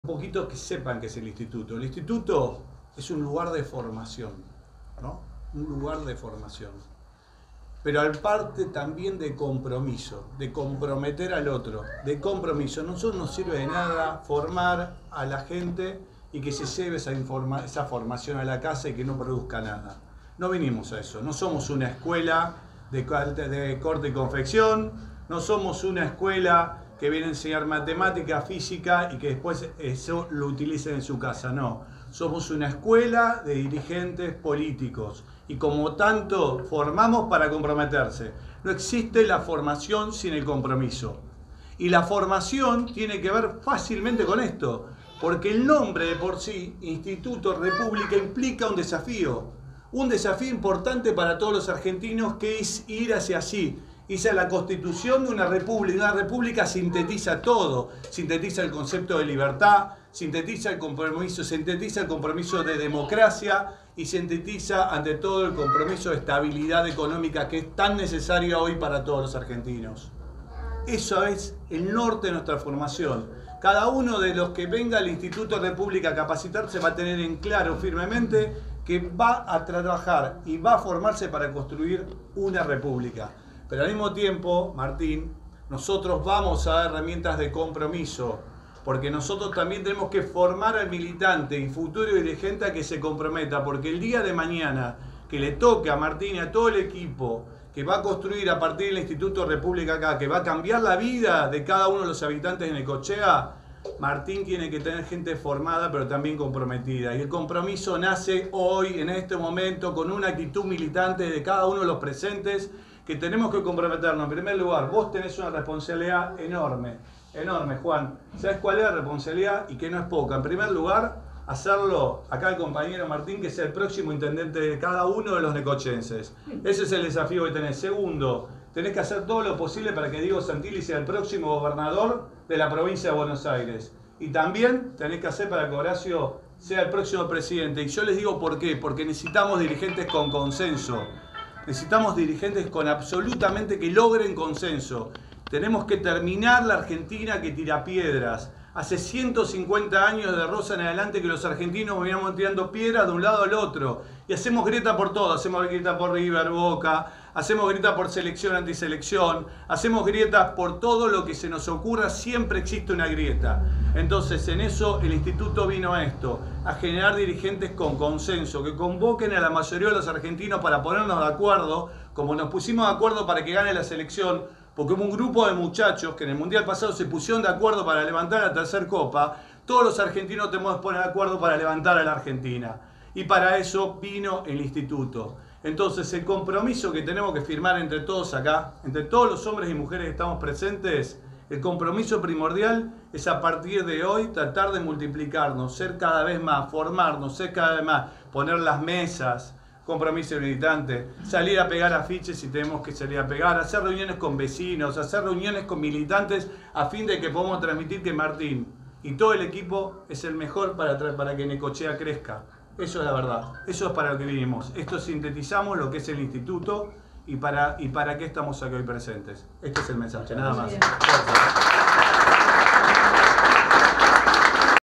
FORMACION Y COMPROMISO
Es lo que pretende el Instituto República, según lo manifestado por su presidente, José Luis Acevedo, al inaugurar la sede de Necochea. Trabajan para las candidaturas de Horacio Rodríguez Larreta (en la Nación), Diego Santilli (en la Provincia) y Martín Migueles (en Necochea)